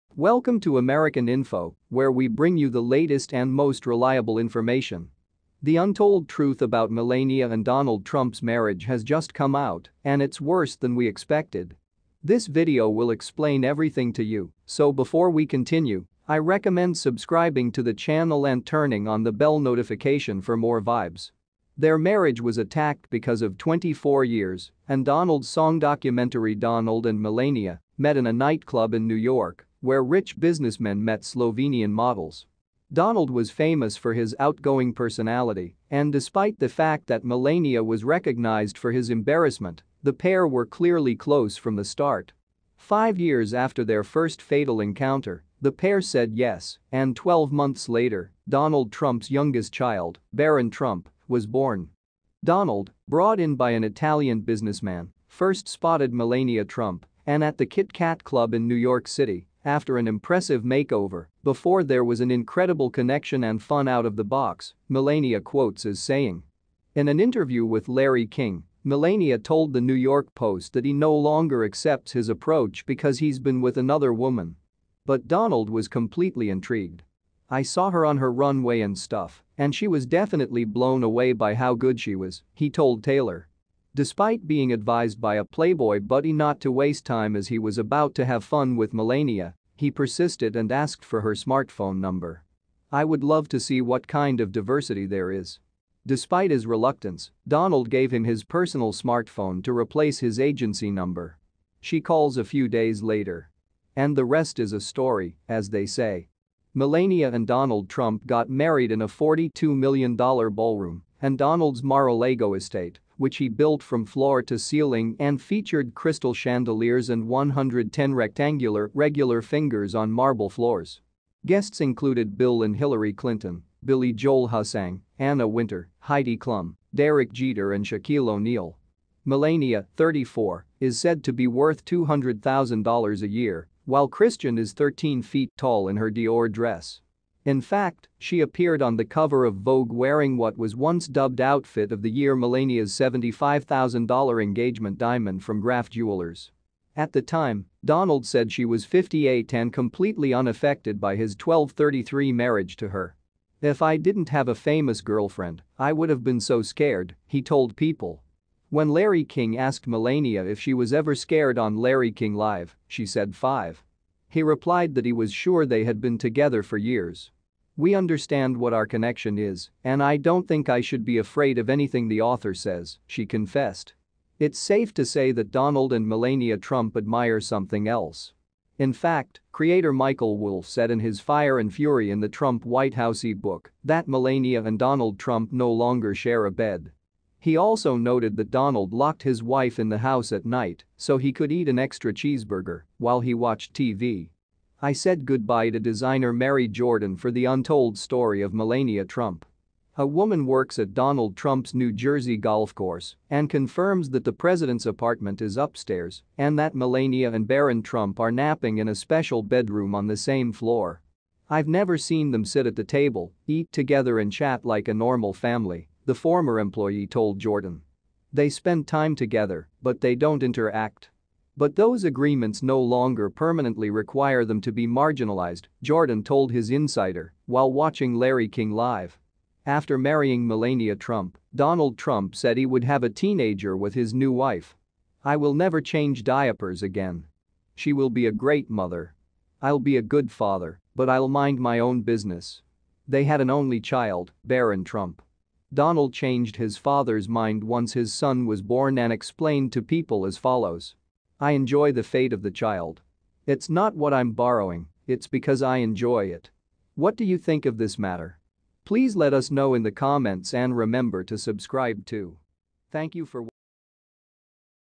Melania sels trump as she hands new stolen FILES to FBI?
Their marriage was attacked because of 24 years and Donald's song documentary Donald and Melania met in a nightclub in New York, where rich businessmen met Slovenian models.